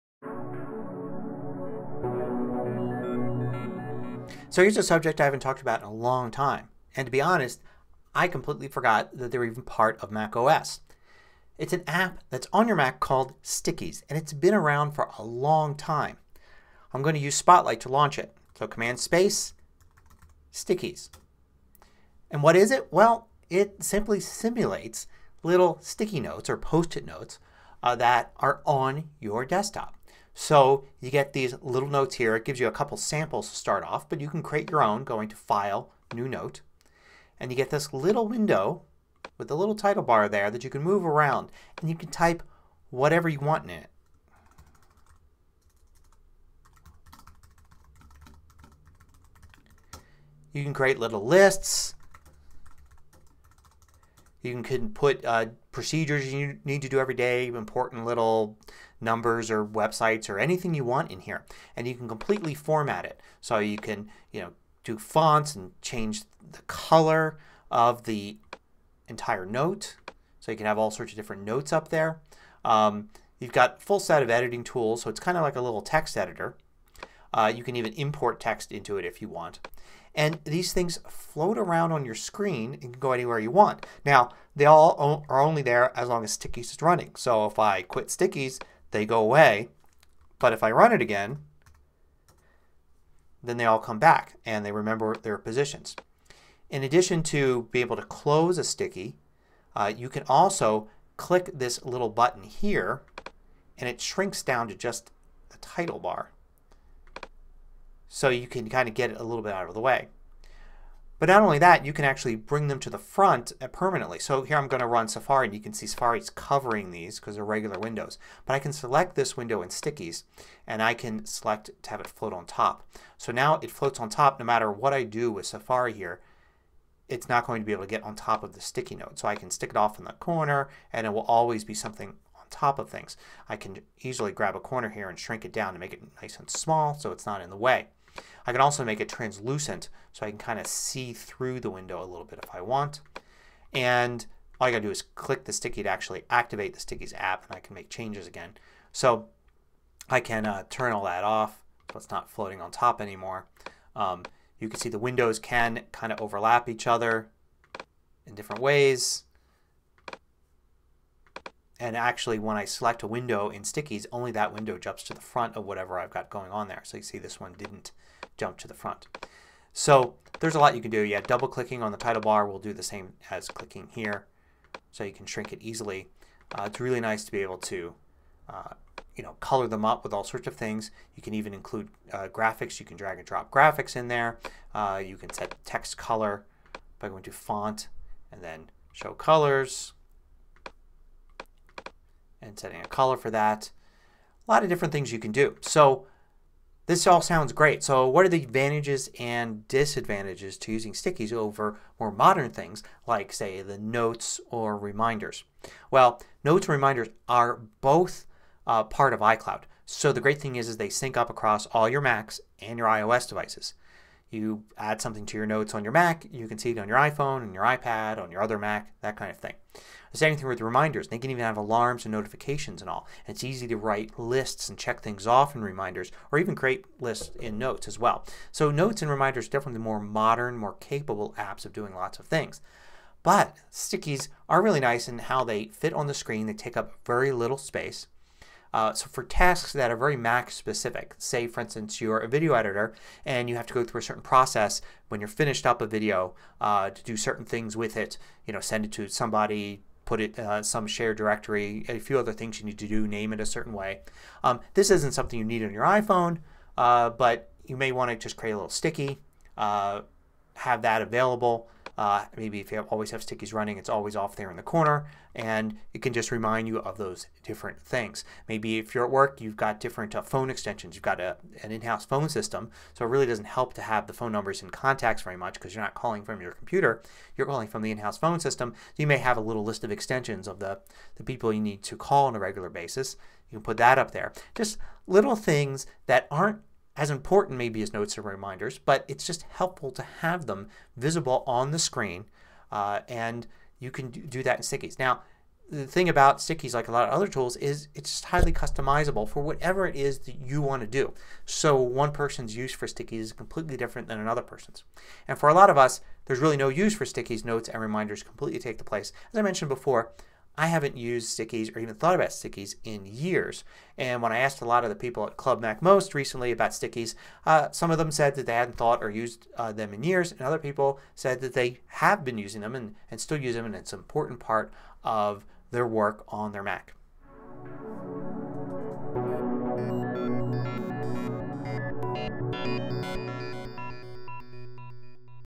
Using Mac Stickies (#1590)
Stickies is an app that has been around on the Mac long before Notes, Reminders and iCloud. You can place little notes on your Desktop to refer to while working. You can even have these notes remain on top of other windows. While it doesn't offer some of the advanced functionality of today's apps, it can still be useful as a Mac-only tool for helping you get work done.